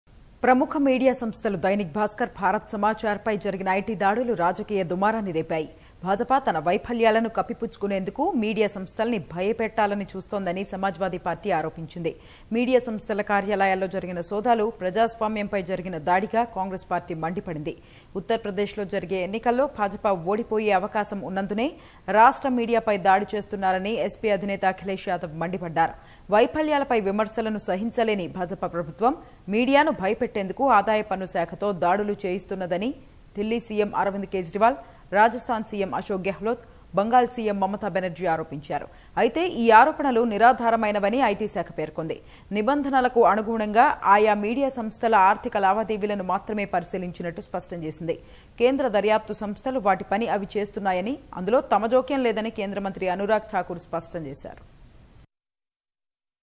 ప్రముఖ మీడియా సంస్థలపై ఐటీ దాడుల్ని ఖండించిన విపక్షాలు | SP, BSP, UP Cong Condemn IT Raids on Media
ప్రముఖ మీడియా సంస్థలు దైనిక్ భాస్కర్ , భారత్ సమాచార్ పై.. జరిగిన ఐటీ దాడులు రాజకీయ దుమారాన్ని రేపాయి. భాజపా. తన వైఫల్యాలను కప్పిపుచ్చుకునేందుకు మీడియా సంస్థల్ని భయపెట్టాలని చూస్తోందని సమాజ్ వాదీ పార్టీ ఆరోపించింది.మీడియా సంస్థల కార్యాలయాల్లో జరిగిన సోదాలు..ప్రజాస్వామ్యంపై జరిగిన దాడిగా కాంగ్రెస్ పార్టీ మండిపడింది. ఉత్తర్ ప్రదేశ్ లో జరిగే ఎన్నికల్లో..భాజపా ఓడిపోయే అవకాశమున్నందునే రాష్ట్ర మీడియాపై దాడి చేస్తున్నారని. ఎస్పీ అధినేత అఖిలేశ్ యాదవ్ మండిపడ్డారు. వైఫల్యాలపై విమర్శలను సహించలేని. భాజపా ప్రభుత్వం మీడియాను భయపెట్టేందుకు ఆదాయ పన్ను శాఖతో దాడులు చేయిస్తున్నదని దిల్లీ సీఎం అరవింద్ కేజ్రీవాల్ ,రాజస్థాన్ సీఎం అశోక్ గెహ్లోత్ , బంగాల్ సీఎం మమత బెనర్జీ ఆరోపించారు. ఐతే. ఈ ఆరోపణలు నిరాధారమైనవని ఐటీ శాఖ పేర్కొంది. నిబంధనల అనుగుణంగా ఆయా మీడియాసంస్థల ఆర్థిక లావాదేవీలను మాత్రమే పరిశీలించినట్లు... స్పష్టం చేసింది. కేంద్ర దర్యాప్తు సంస్థలు వాటి పని అవి చేస్తున్నాయని. అందులో తమ జోక్యం లేదని కేంద్ర మంత్రి అనురాగ్ ఠాకూర్ స్పష్టం చేశారు.